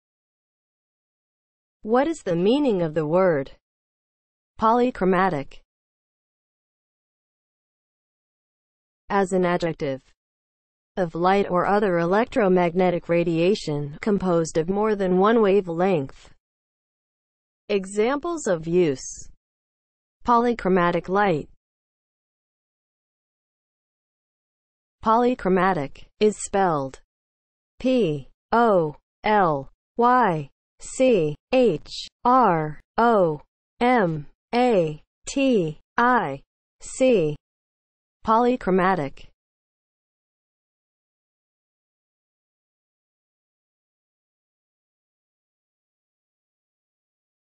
What is the meaning of the word POLYCHROMATIC?
Definition, examples of use and spelling of the word POLYCHROMATIC.

(00:00) Intro
(00:08) As an adjective
(00:23) Spelling

As an adjective (00:08):
• (of light or other electromagnetic radiation) composed of more than one wavelength

Examples:
• "polychromatic light"